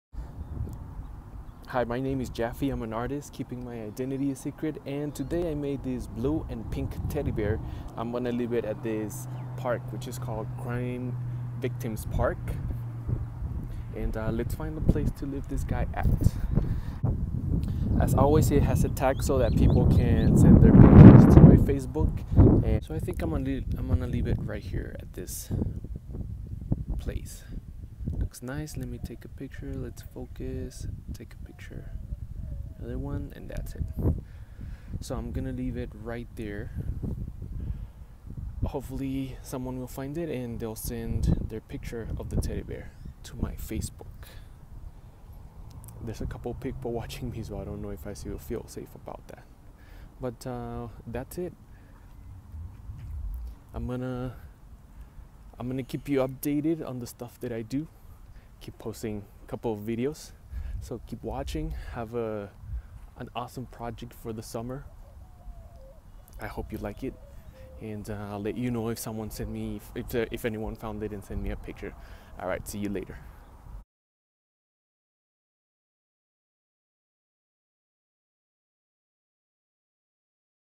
Artist leaves teddy bears on the street every day - vlog 12 El Paso Tx
Wanna buy my work?
Artista en la Ciudad de Juárez
Street artist cd juarez
El paso artist